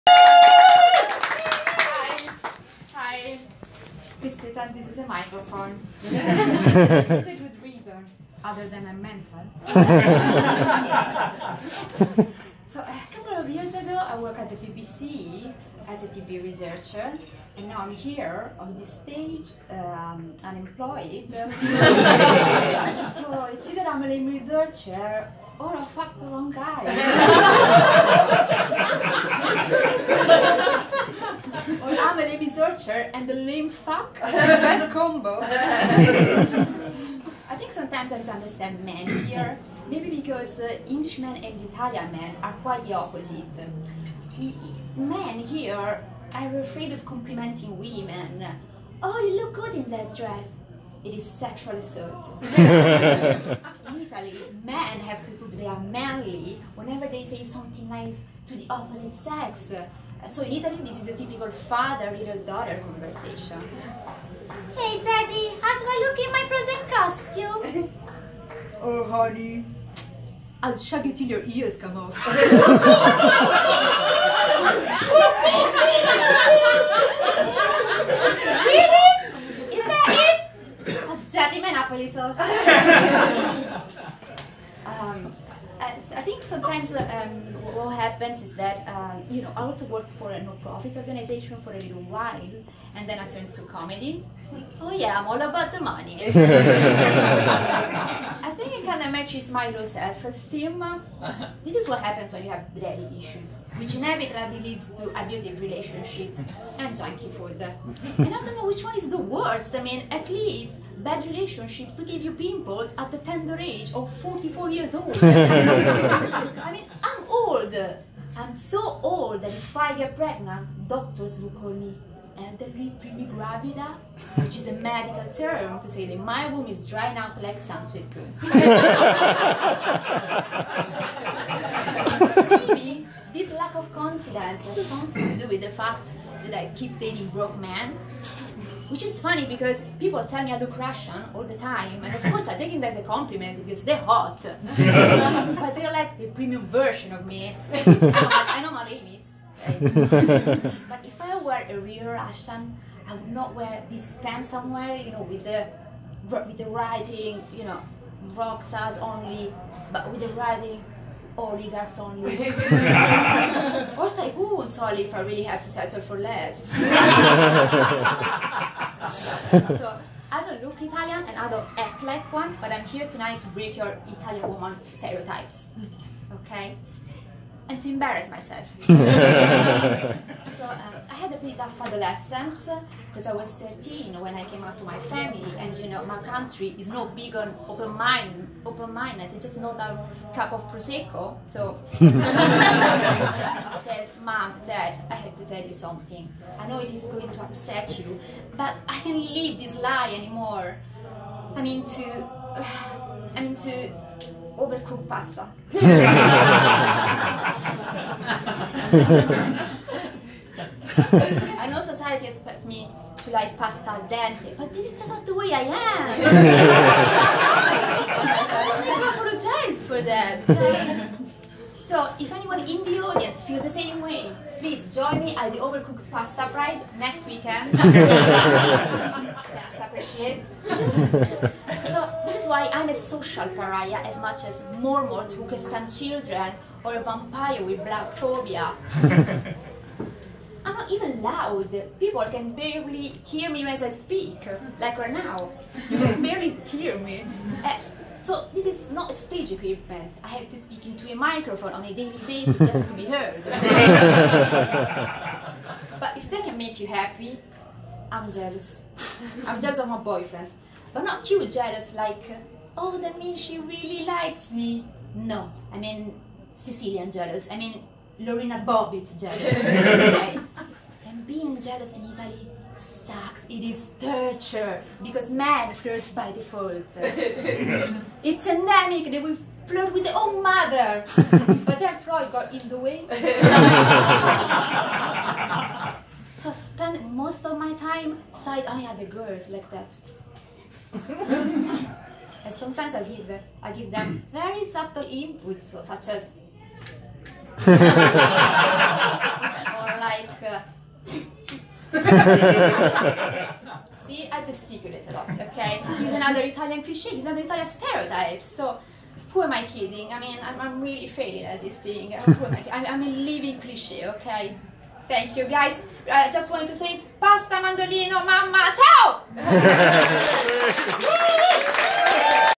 stand-up comedy: ITALIAN STEREOTYPES
My first gig in English in a London pub